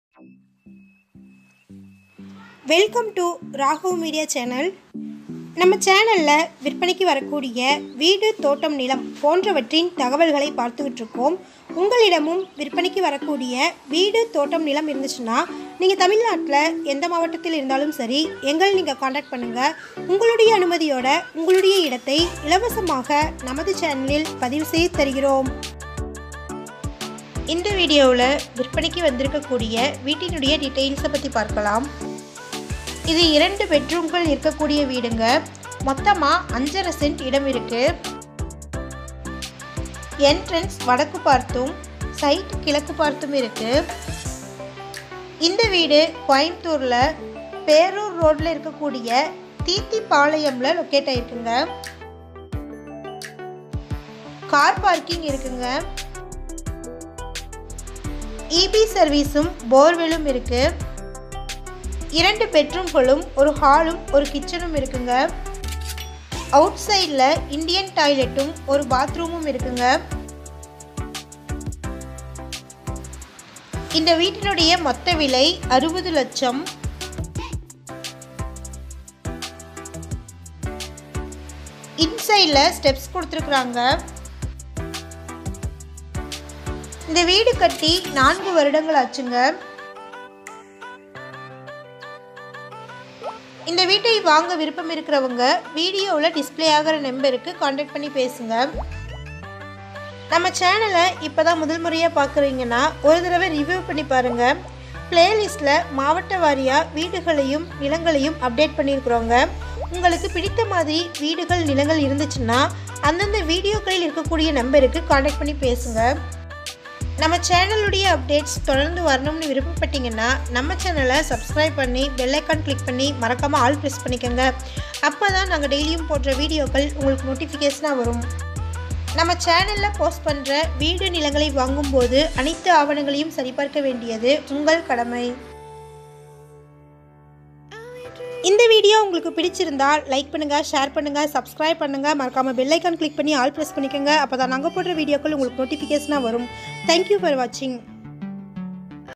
இரண்டு படுக்கை அறை உள்ள வீடு விற்பனைக்கு/2BHK Individual House For Sale/Ragav Media Real Estate
HOUSE SALE
PERUR ROAD
THEETHIPALAYAM

60LAKH

2BHK+2BATHROOM

5.5CENT

4-YEARS BUILDING

NORTH FACE ENTRANCE

EAST FACE SITE

EB AVAILABLE

BORE AVAILABLE

OUTSIDE
1-INDIAN TOILET
1-BATHROOM

GANESAN S
MEDIATOR